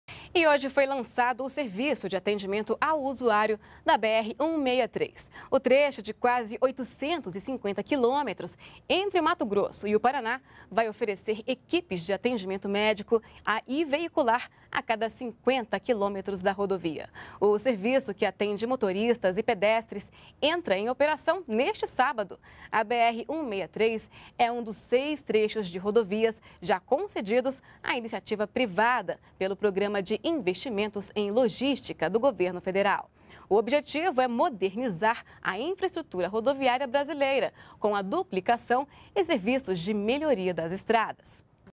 Lançado serviço de atendimento ao usuário na BR 163
NBR NOTÍCIAS - 09.10.14: Nesta quinta-feira (9), foi lançado o serviço de atendimento ao usuário na BR 163. O trecho entre Mato Grosso e Paraná vai oferecer equipes de atendimento médico e veicular a cada 50 quilômetros da rodovia. O objetivo é modernizar a infraestrutura rodoviária brasileira com a duplicação e serviços de melhoria das estradas. O serviço entra em operação a partir deste sábado (11).